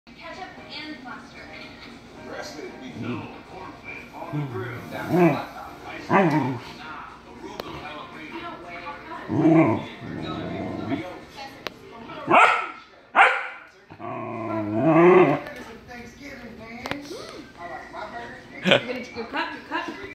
Irish terrier
Sees own reflection gets excited